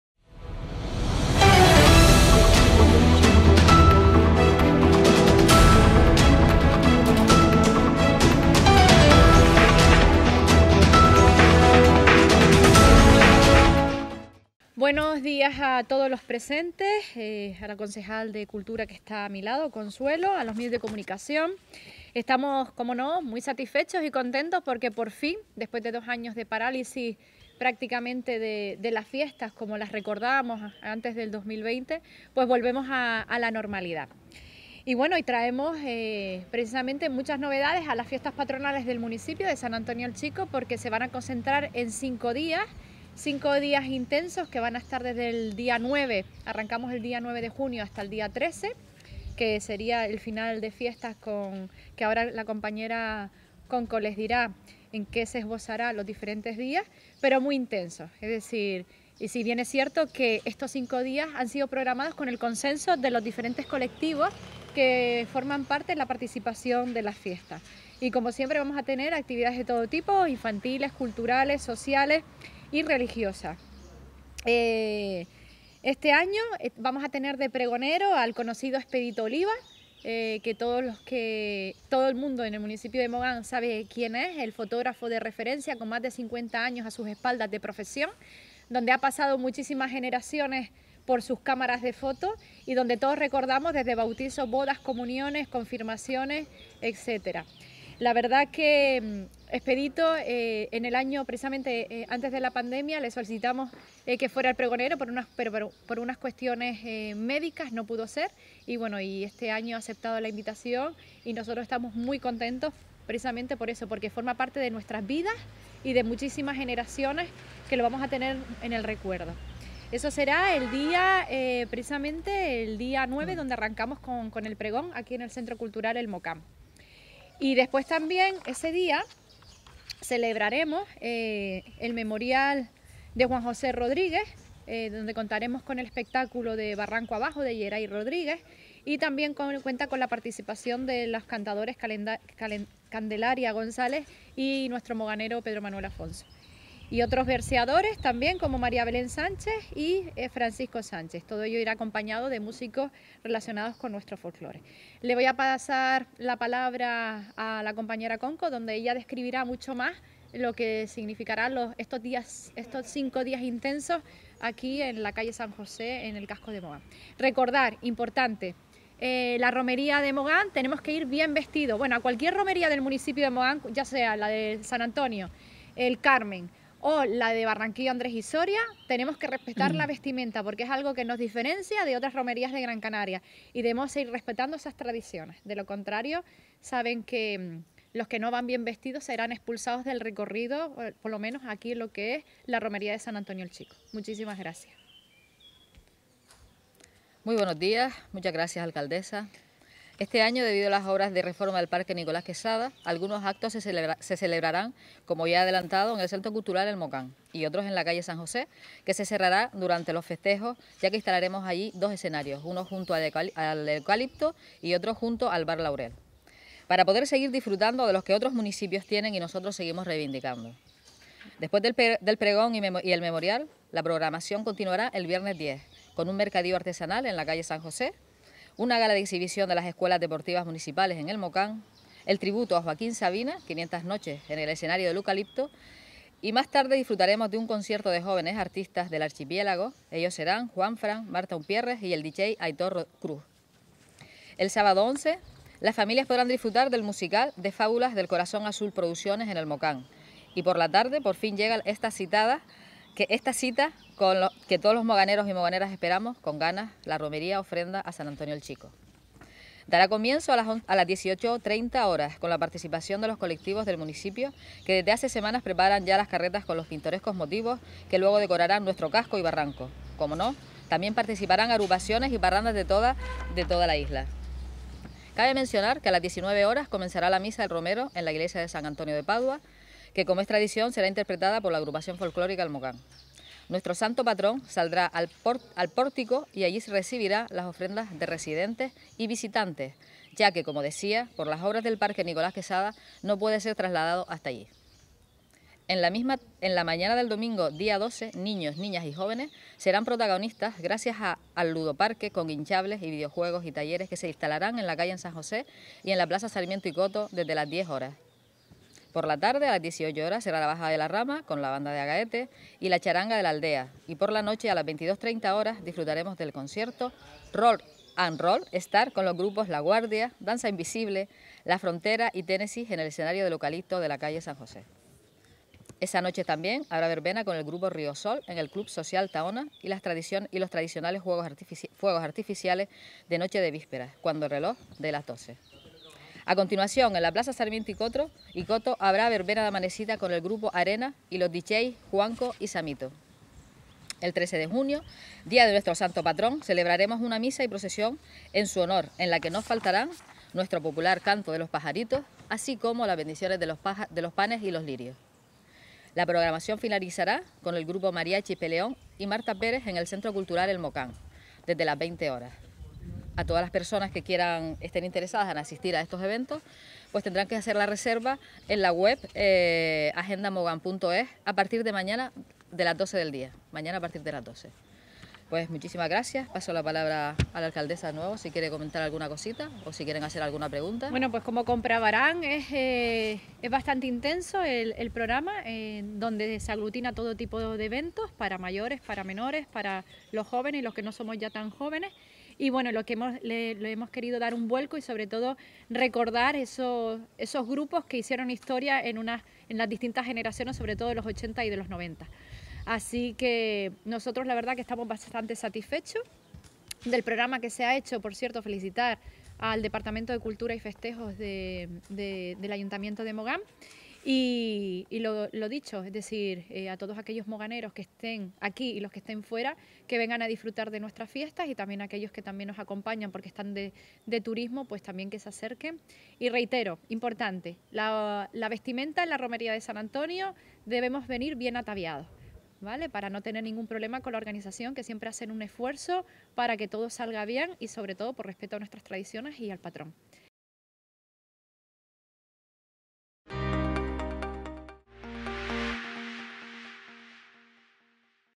Presentación de las Fiestas Patronales San Antonio El Chico 2022
Presentación de las Fiestas Patronales San Antonio El Chico 2022 que se celebran del 9 al 13 de junio